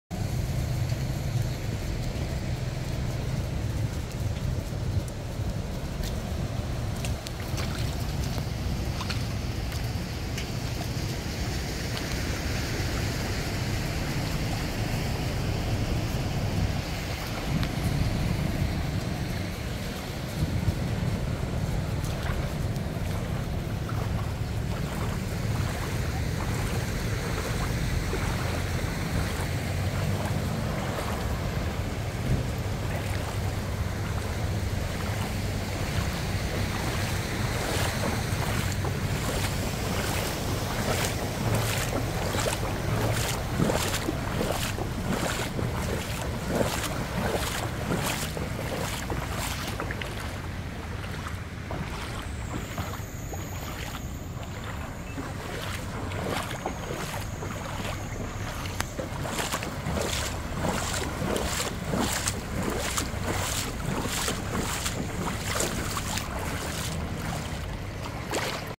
马鲁帝部分街道淹水
大选来了，大水也来了！

马鲁帝今早部分街道受淹，水深约半呎。

根据气象局预测，11月19日投票日，国内大部分地区会下雨，这更让镇民担心水患加剧，出入不便。